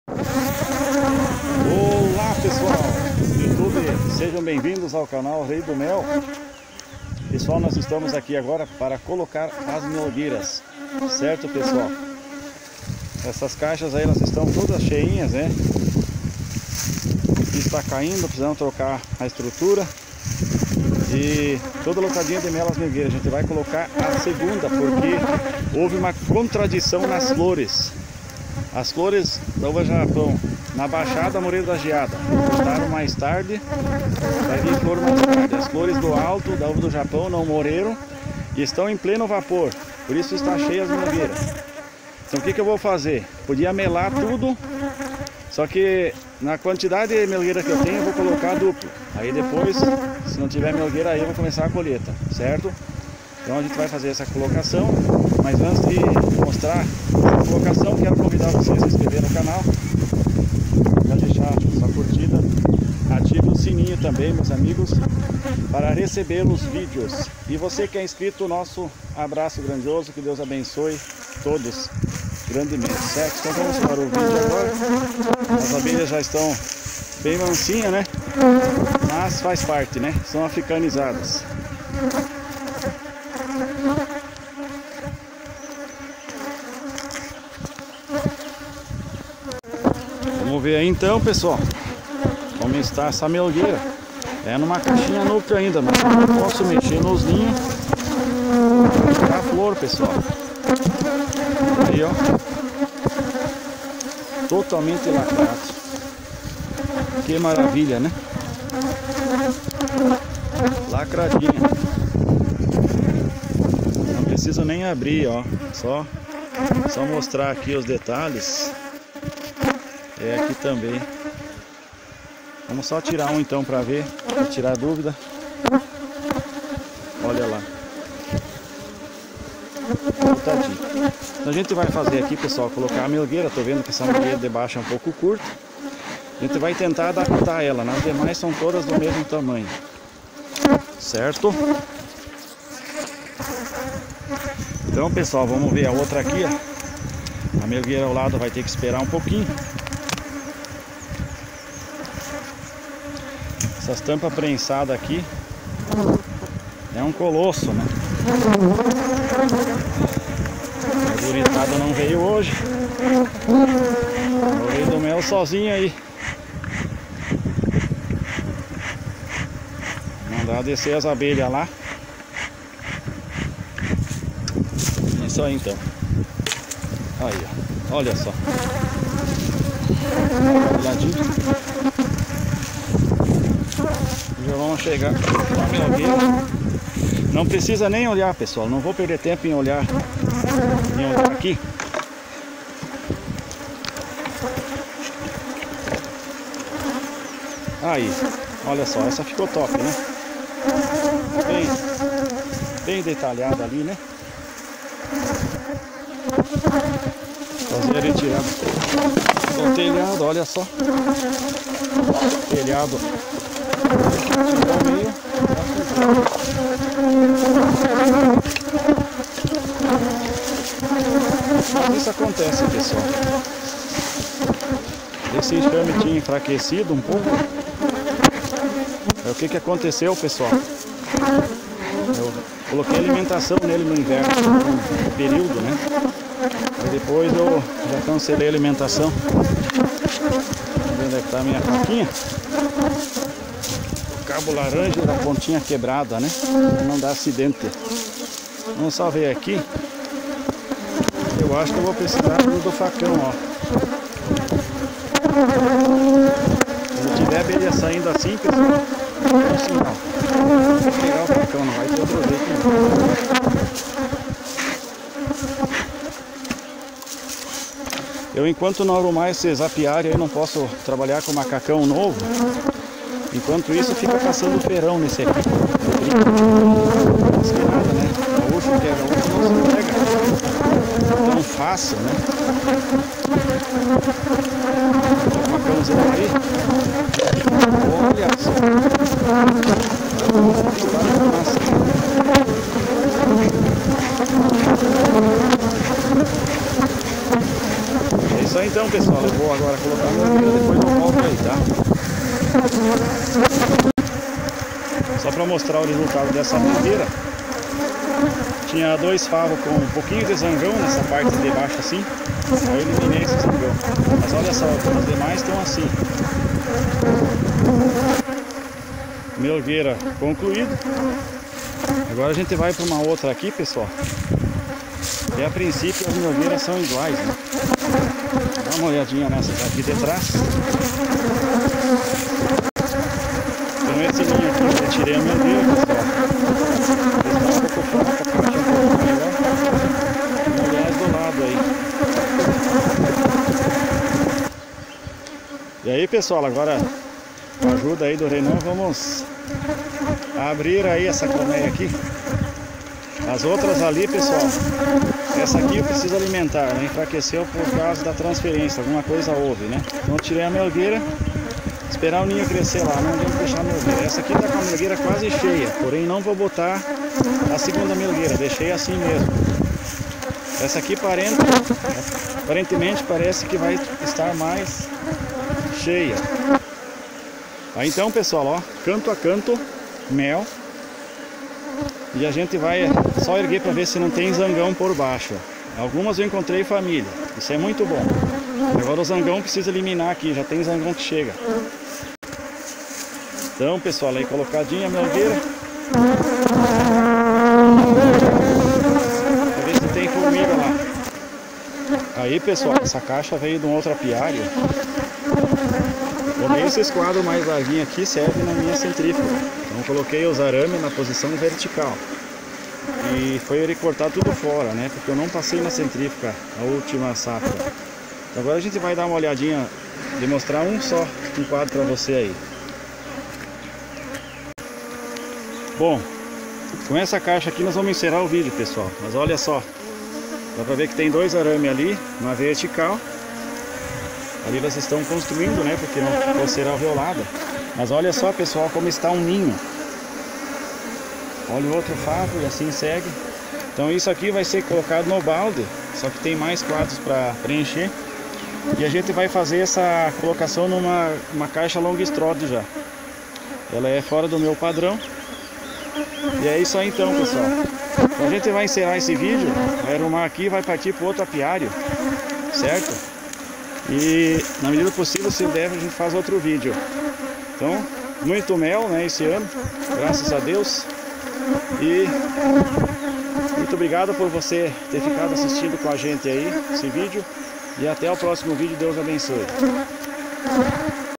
SEGUNDA MELGUEIRA (MANEJO)
Algumas melgueiras deram um pouco de trabalho para colocar mas no final deu tudo certo